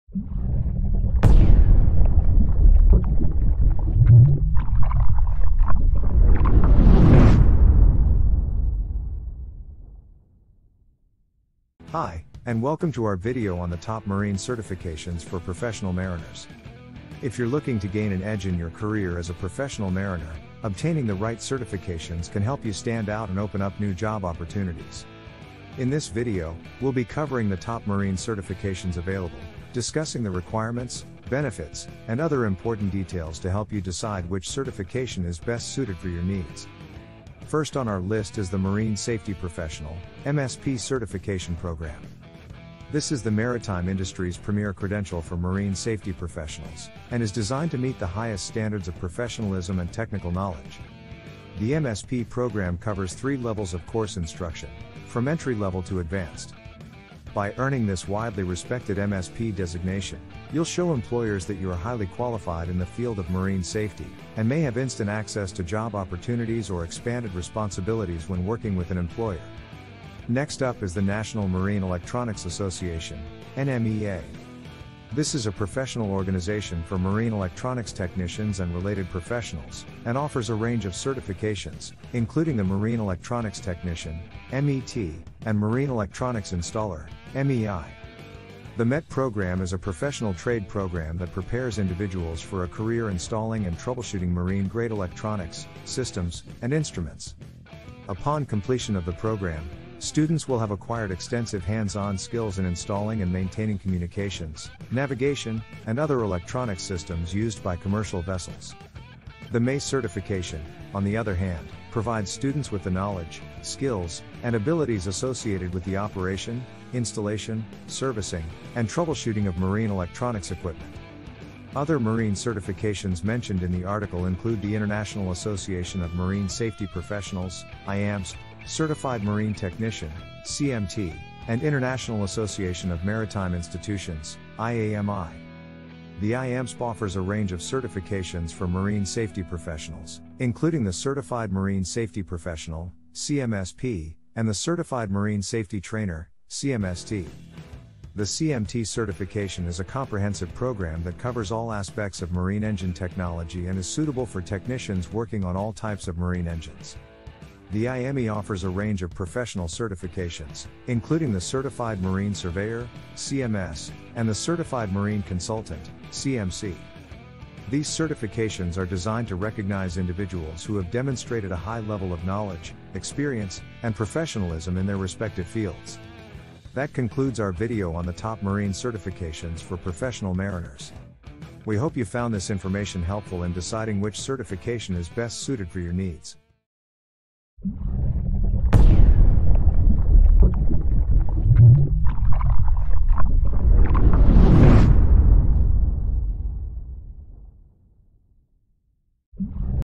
The Top Marine Certifications for Professional Mariners
Are you a professional mariner looking to gain an edge in your career? Obtaining the right certifications can help you stand out and open up new job opportunities. From basic qualifications to more advanced certifications, there is something for everyone. In this article, we will cover the top marine certifications available for professional mariners. We will discuss the requirements, benefits and other important details that will help you decide which certification is best suited for your needs.